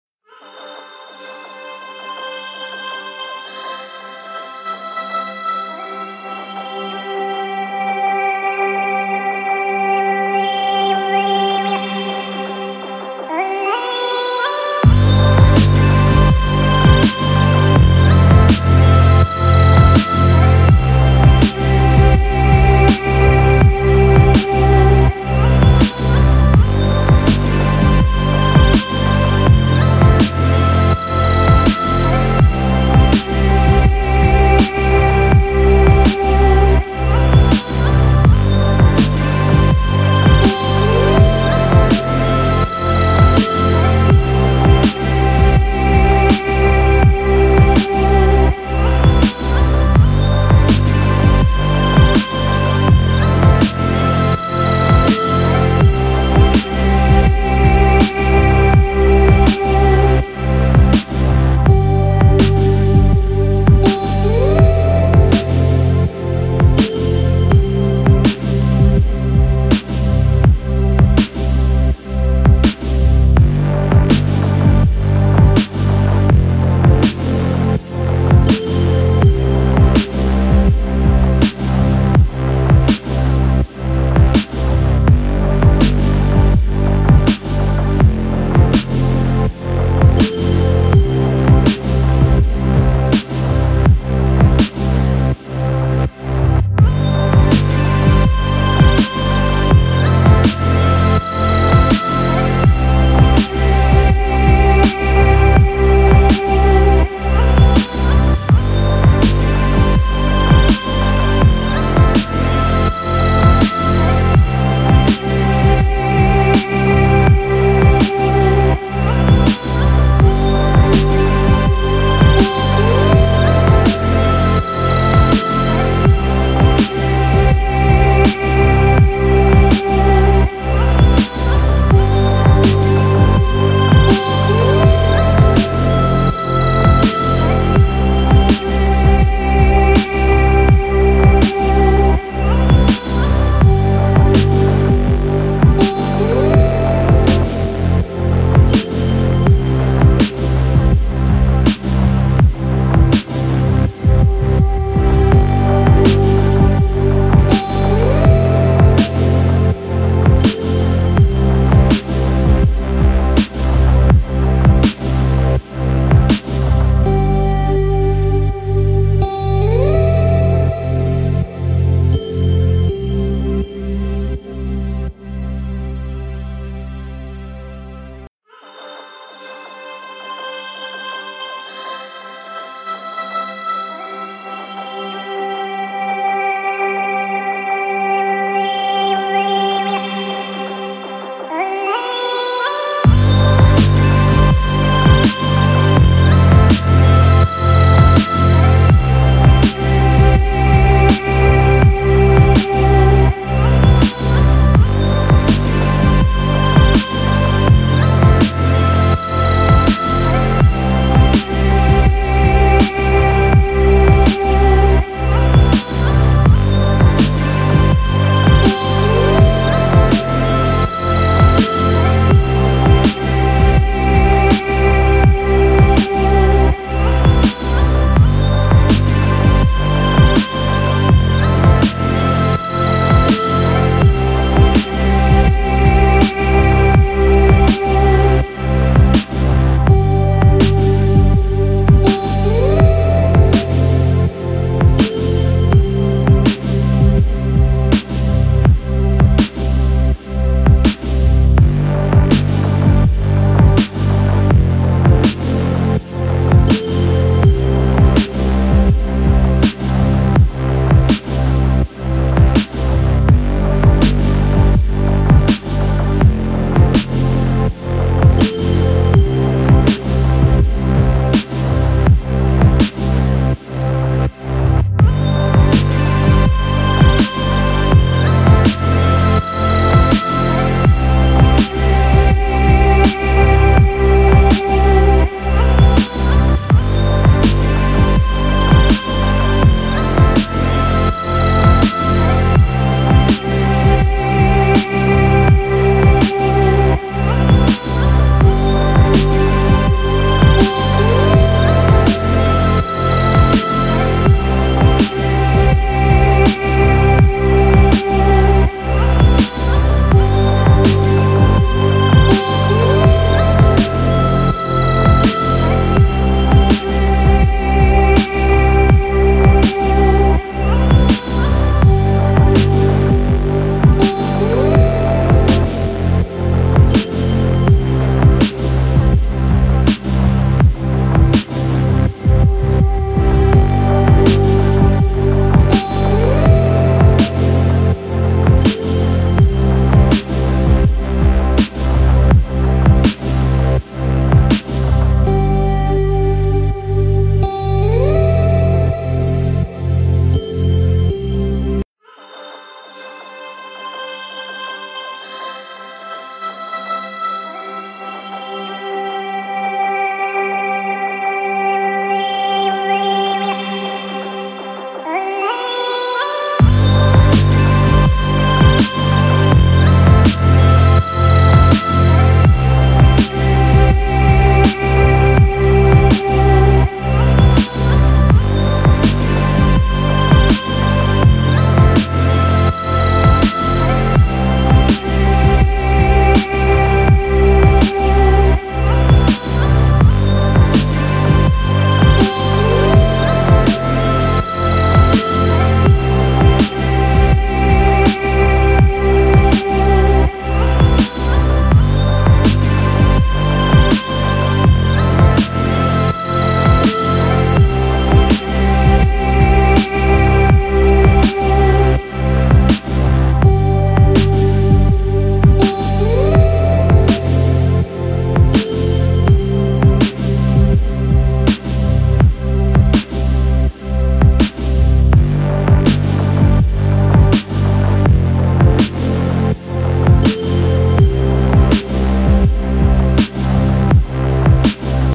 typhoon H at old cotton mill
old cotton mill   erected AD 1872 , abandoned in 1954  /       , someone is renting out some floor space today ; north grosvernordale section of Thompson ct. (so cool)